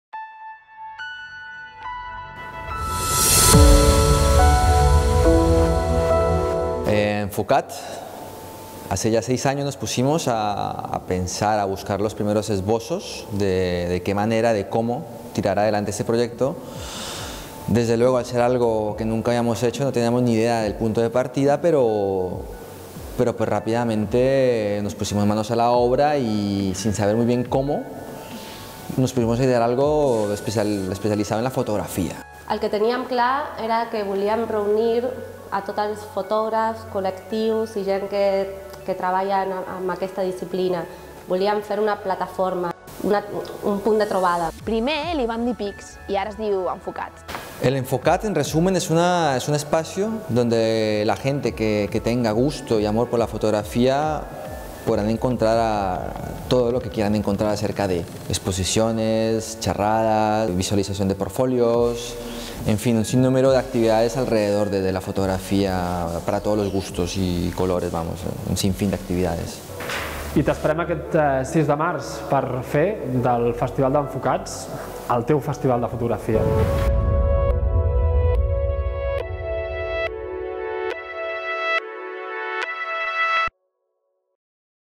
Enfocats 2015, el festival de fotografia del Pati Llimona
Del 6 de març al 4 d'abril, t'oferim de l'acció fotogràfica a la fotografia social, amb tallers, activitats i exposicions. Per a que l'Enfocats sigui el teu festival de fotografia